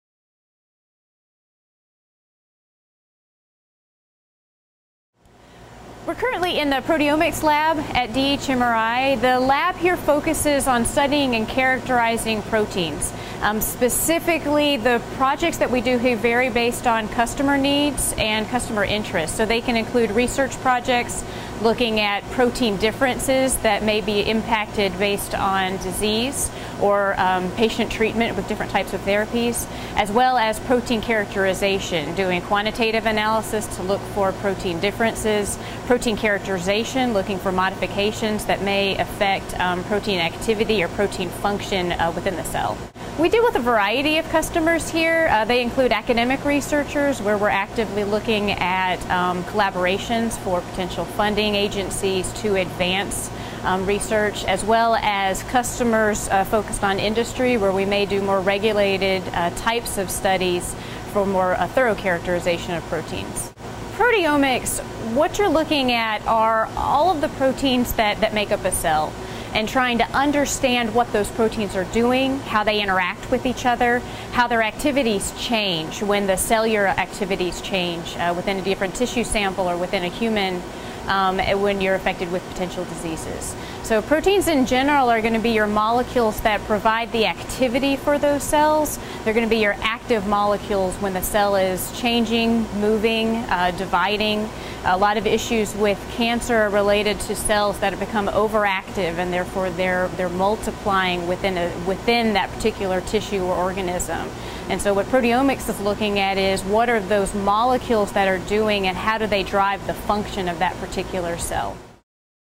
DHMRI Proteomics Lab in Kannapolis
DHMRI Proteomics Lab Manager Sarah Schwartz walks viewers through the Proteomics lab at the DHMRI on the NC Research Campus in Kannapolis.